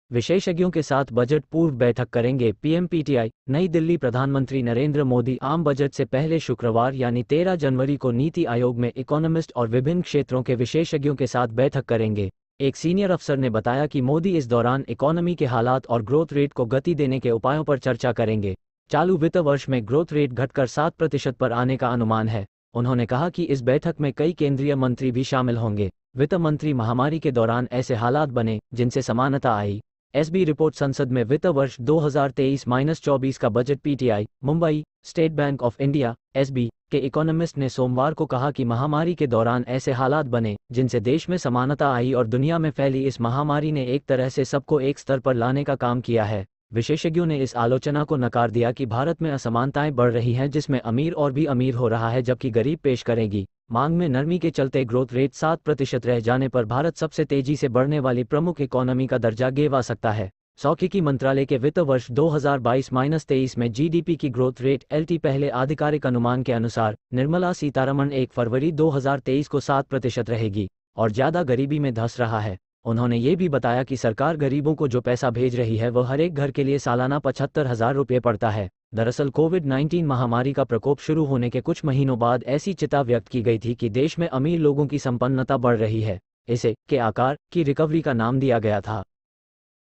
विशेषज्ञों के साथ बजट पूर्व बैठक करेंगे पीएमपीटीआई, नई दिल्लीप्रधानमंत्री नरेंद्र मोदी आम बजट सेपहल
विशेषज्ञों के साथ बजट पूर्व बैठक करेंगे पीएम
पीटीआई, नई दिल्ली
प्रधानमंत्री नरेंद्र मोदी आम बजट से
पहले शुक्रवार यानी 13 जनवरी को नीति
आयोग में इकॉनमिस्ट और विभिन्न क्षेत्रों
के विशेषज्ञों के साथ बैठक करेंगे। एक
सीनियर अफसर ने बताया कि मोदी इस
दौरान इकॉनमी के हालात और ग्रोथ रेट को
गति देने के उपायों पर चर्चा करेंगे। चालू
वित्त वर्ष में ग्रोथ रेट घटकर सात प्रतिशत
पर आने का अनुमान है।
उन्होंने कहा कि इस बैठक में कई
केंद्रीय मंत्री भी शामिल होंगे। वित्त मंत्री
महामारी के दौरान ऐसे हालात बने,
जिनसे समानता आई: SB
रिपोर्ट
संसद में वित्त वर्ष 2023-24 का बजट
पीटीआई, मुंबई: स्टेट बैंक
ऑफ इंडिया (SB) के इकॉनमिस्ट ने
सोमवार को कहा कि महामारी के दौरान
ऐसे हालात बने, जिनसे देश में समानता
आई और दुनिया में फैली इस महामारी
ने एक तरह से सबको एक स्तर पर
लाने का काम किया है। विशेषज्ञो ने इस
आलोचना को नकार दिया कि भारत में
असमानताएं बढ़ रही हैं जिसमें अमीर
और भी अमीर हो रहा है जबकि गरीब
पेश करेंगी। मांग में नरमी के चलते ग्रोथ
रेट सात प्रतिशत रह जाने पर भारत सबसे
तेजी से बढ़ने वाली प्रमुख इकॉनमी का
दर्जा गेवा सकता है। सॉख्यिकी मंत्रालय के
वित्त वर्ष 2022-23 में GDP की ग्रोथ रेट
LT
पहले आधिकारिक अनुमान के अनुसार,
निर्मला सीतारमण 1 फरवरी, 2023 को सात प्रतिशत रहेगी।
और ज्यादा गरीबी में धंस रहा है।
उन्होंने यह भी बताया कि सरकार गरीबों
को जो पैसा भेज रही है वह हरेक घर
के लिए सालाना 75000 रुपये पड़ता
है। दरअसल कोविड-19 महामारी का
प्रकोप शुरू होने के कुछ महीनों बाद
ऐसी चिता व्यक्त की गई थी कि देश में
अमीर लोगों की सम्पन्नता बढ़ रही है।
इसे 'के-आकार' की रिकवरी का नाम
दिया गया था।